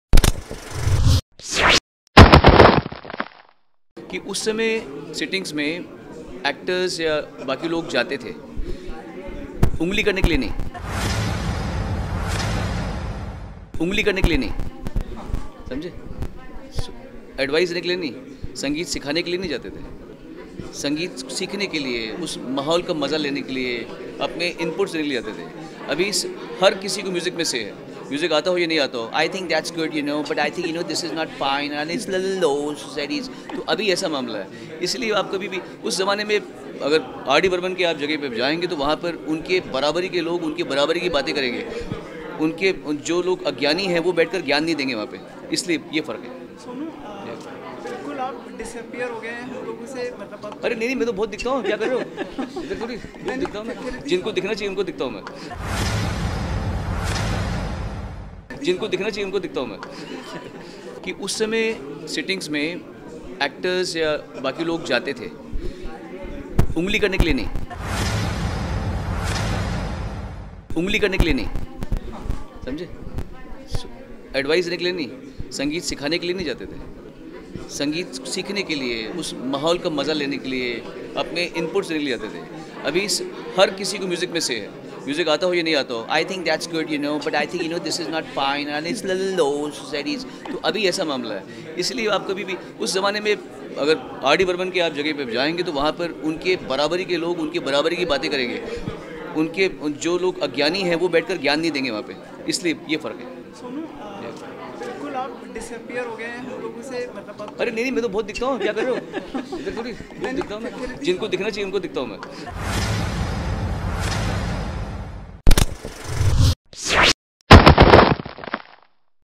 सोनू निगम ने उड़ाया सलमान खान का मज़ाक, विडियो देख कर चौंक जाओगे
Sonu Nigam makes fun of Salman Khan's Singing.